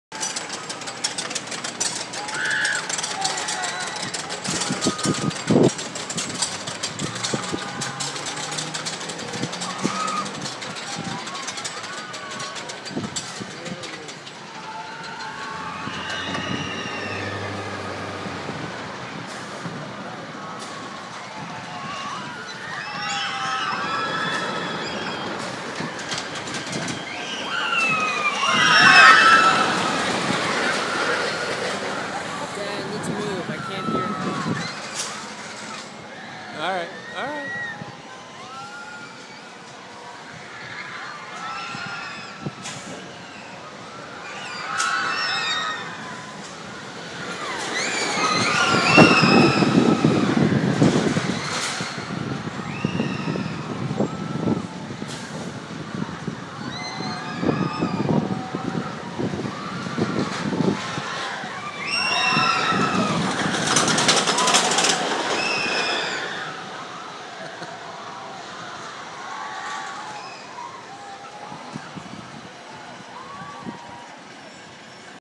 The Giant Dipper
Built in 1924 the Giant Dipper is a National Landmark.  A classic roller coaster in Santa Cruz, CA.